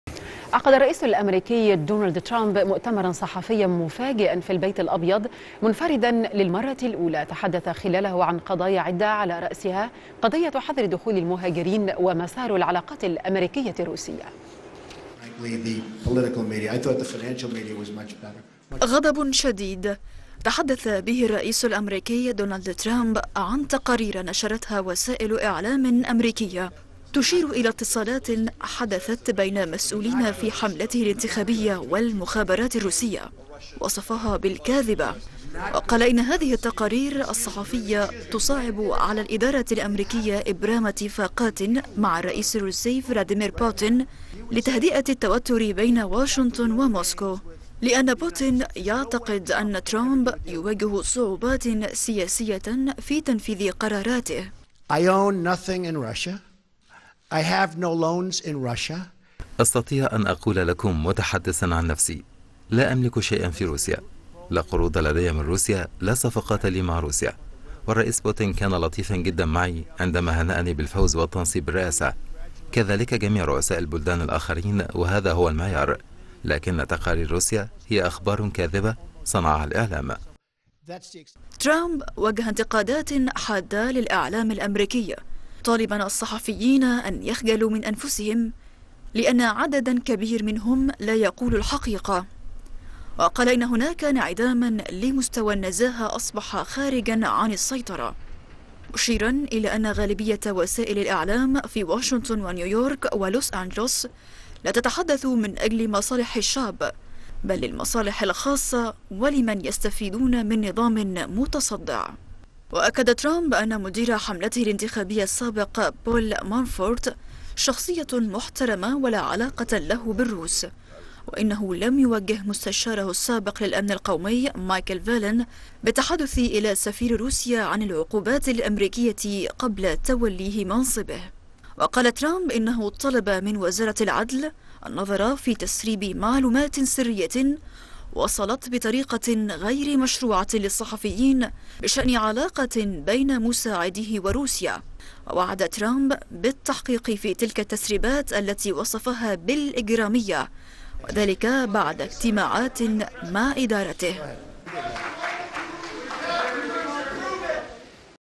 خلال مؤتمر صحفي.. ترامب يعرب عن غضبه من الإعلام الأمريكي
الموقع الرسمي للقناة  ..